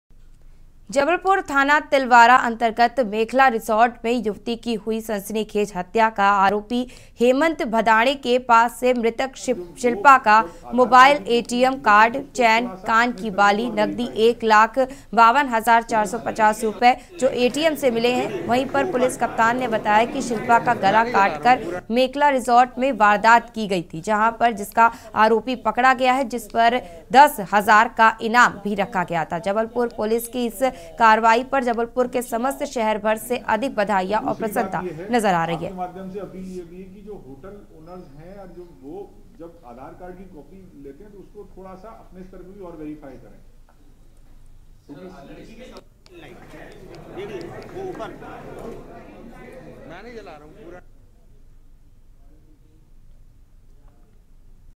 Jabalpur : पुलिस को मिली सफलता, शिल्पा मर्डर केस का आरोपी हुआ गिरफ्तार || K News India
जबलपुर थाना  तिलवारा अंतर्गत मेखला रिसोर्ट में युवती की हुई सनसनीखेज हत्या का आरोपी हेमंत भदाणे  के पास से मृतक शिल्पा का मोबाइल ए.टी.एम कार्ड, चैन, कान की बाली, नगदी 1 लाख 52 हजार 450 रूपये जो एटीएम से मिला है...वहीं पुलिस कप्तान ने बताया कि शिल्पा का गला काटकर मेकला रिसोर्ट में वारदात की गई... जिसका आरोपी पकड़ा गया है... जिस पर ₹10000 का इनाम भी रखा गया था...